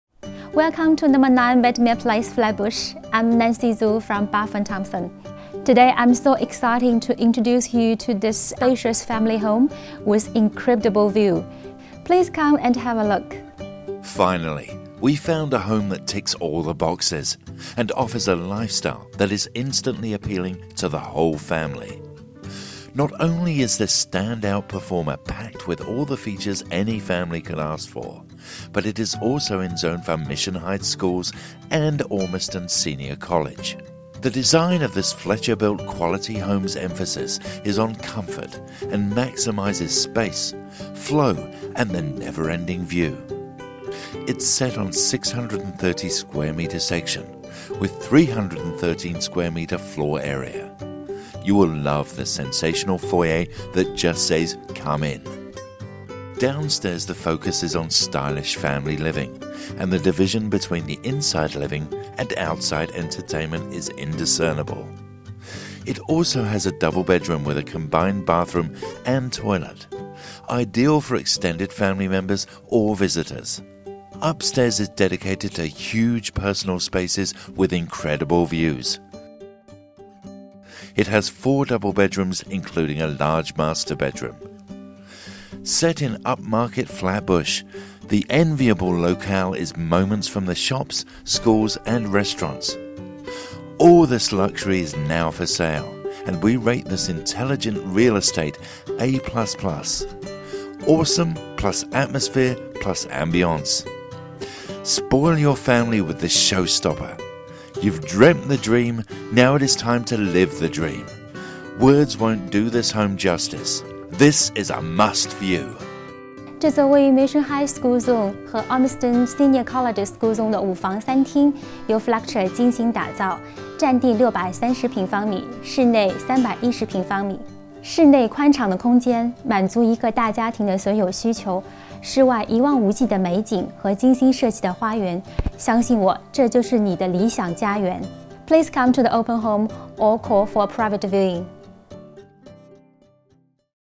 SOLD - 9 Bendemeer Place, Flat Bush - Nancy Zhu
Quality, Space & Views

Designed with today's privileged lifestyle in mind

Finally we found a home that ticks all the boxes and offers a lifestyle that is instantly appealing to the whole family. Not only is this ...

Property Marketed by: Nancy Zhu, Howick